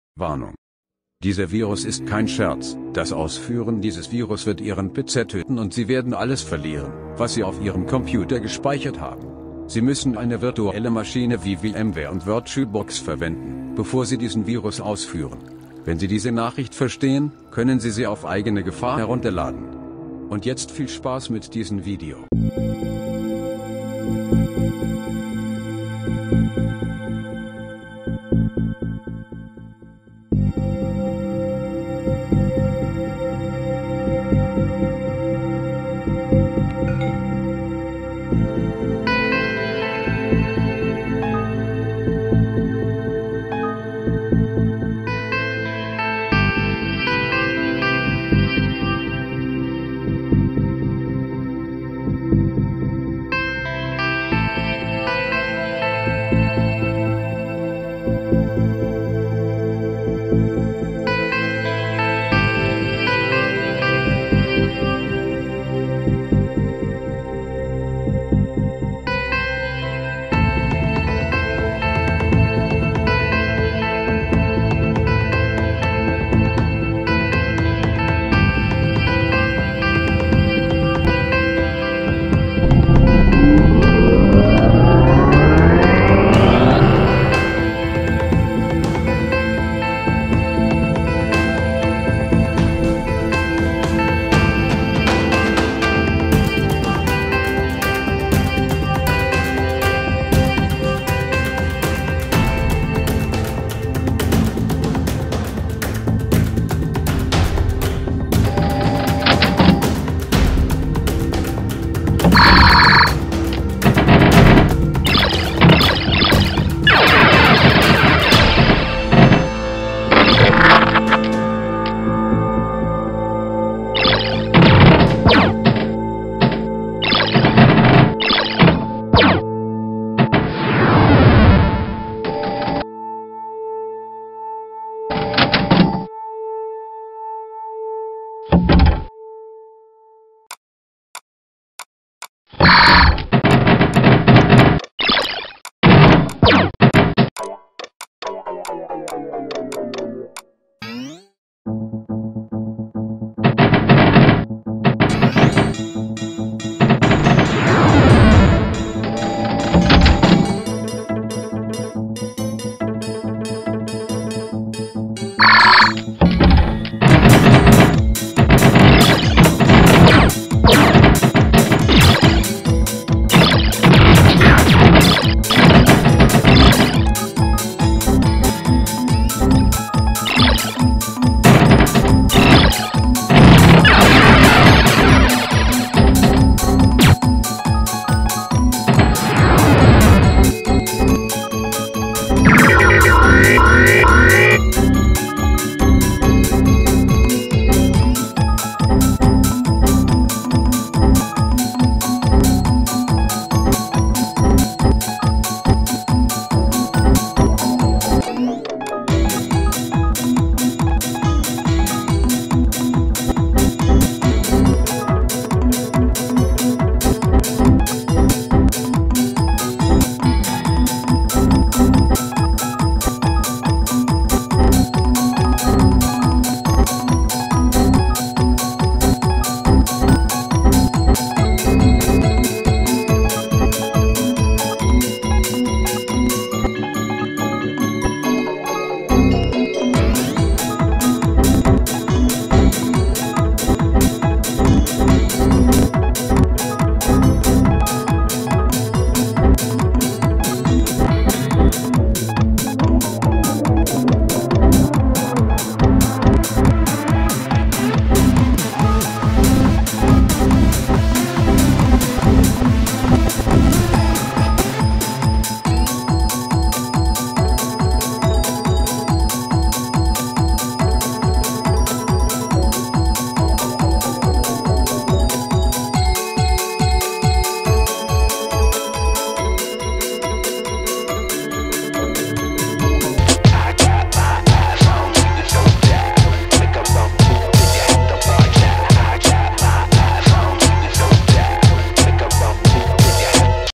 MEMZ.exe On Windows XP
Heute in diesen video Ein Virus der den VM Kill und Der Ist MEMZ Virus

Dieser Virus ist kein Scherz, das Ausführen dieses Virus wird Ihren PC töten und Sie
werden alles verlieren, was Sie auf Ihrem Computer gespeichert haben! Sie müssen
eine virtuelle Maschine wie VMware und Virtualbox verwenden, bevor Sie diesen Virus
ausführen! Wenn Sie diese Nachricht verstehen, können Sie sie auf eigene Gefahr herunterladen!

Der Link: kommen Noch :)